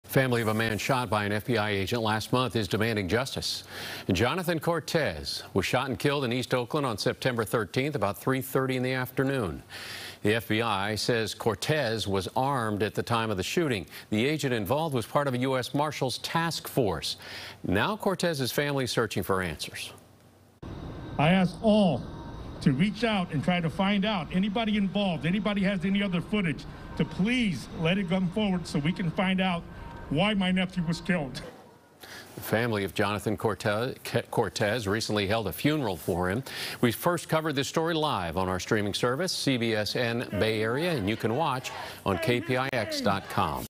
Family of Man Shot in Oakland by FBI Agent Calls for Justice
Allen Martin reports on the family of a man shot by the FBI in Oakland holding a rally demanding answers (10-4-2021)